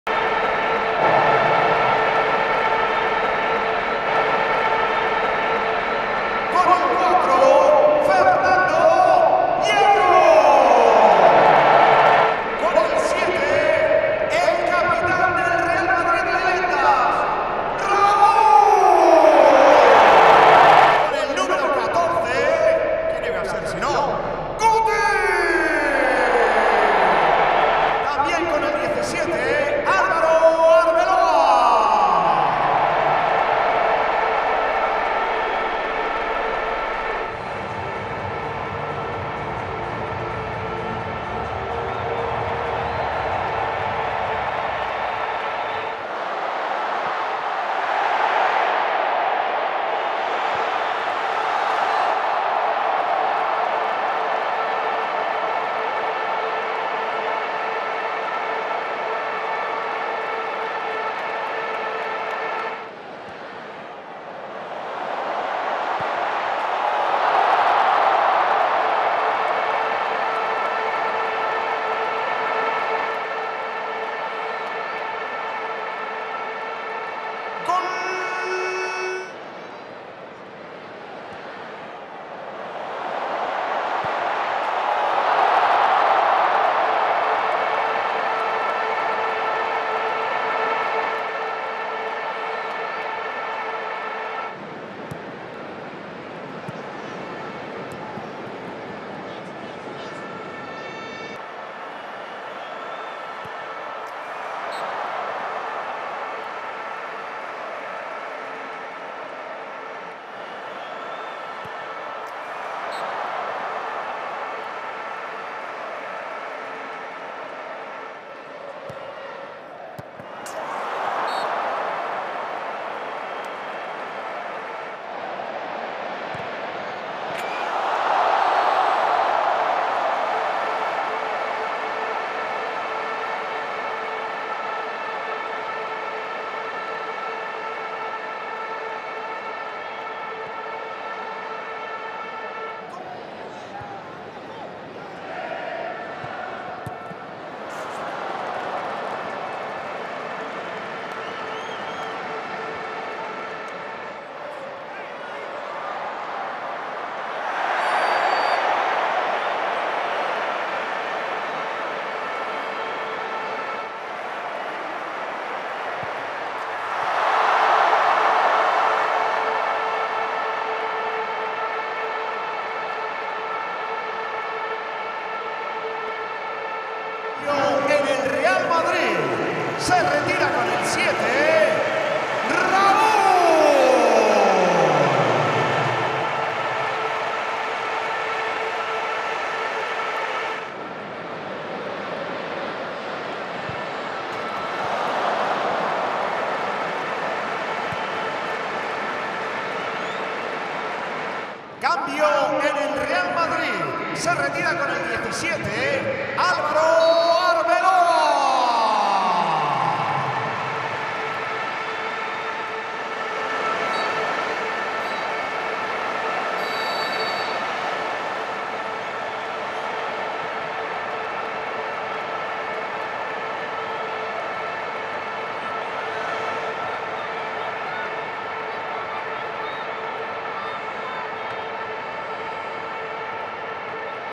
CORAZÓN CLASSIC MATCH | Real Madrid Leyendas 5-4 Chelsea Legends
Relive the goals and highlights from the Corazón Classic Match between Real Madrid Leyendas and Chelsea Legends. Raúl and Fernando Morientes both scored two goals while Christian Karembeu also found the back of the net in the 5-4 victory at the Santiago Bernabéu!